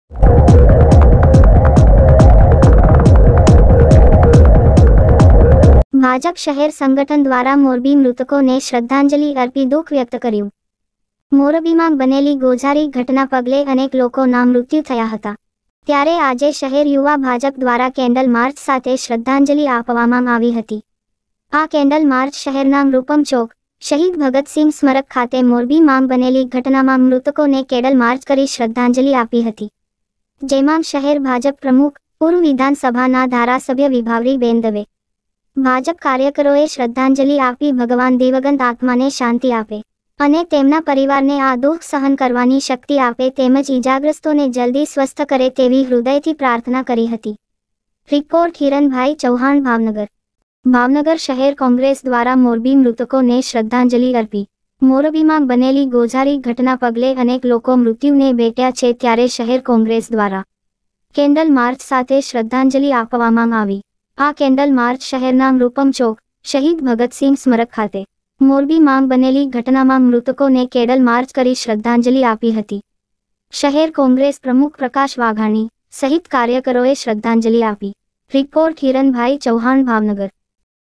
મોરબીની ઘટનાને લઈ ભાવનગરમાં ભાજપ, કોંગ્રેસ દ્વારા આજે........
આગેવાનો પદાધિકારીઓ ઉપસ્થિત